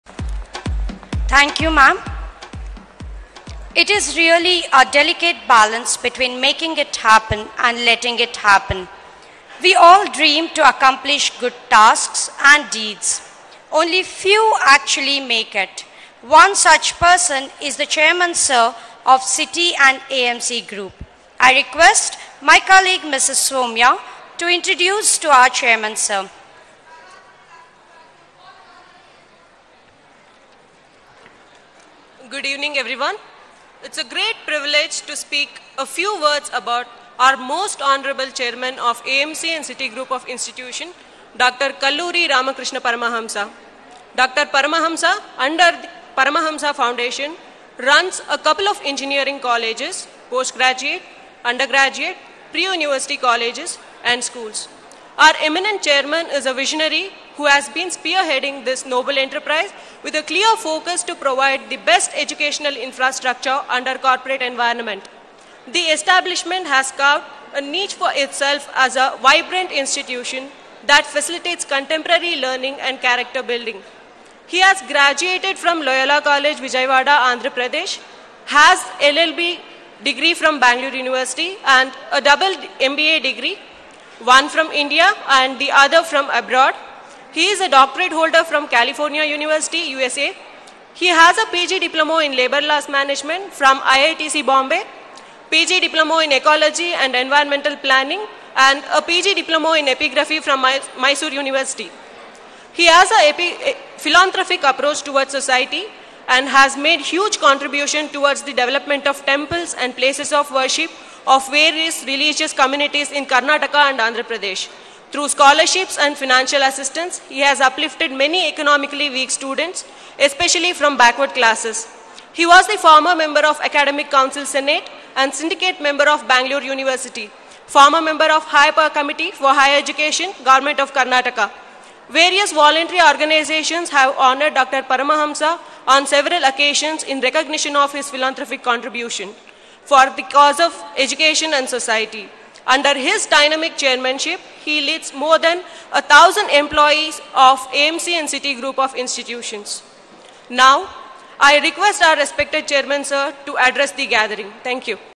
Introduction of Beloved Dr. K.R. Paramahamsa
City College Jayanagar celebrated annually to commemorate the foundation day of the Institute on its premises, with the usual galore and enthusiasm. The day is celebrated at the close of the academic session. It provides an opportunity to assess the achievements and shortfalls. The students are actively associated in all the segments of the programme. Rather, they manage all the activities by themselves.

The presentation of annual report of college activities is a special feature of the annual day function. Felicitation and distribution of certificates of merit in due acknowledgement to the students, faculty and staff of the college for their exemplary performance in different academic pursuits forms another segment of the programme.

The function began with the ceremonial lamp lighting by the dignitaries. Eminent scholars and senior executives were are invited to deliver a talk on the occasion. Following this was a remarkable performances by students who exhibited their hidden talents through cultural extravaganza which was admired by all audience.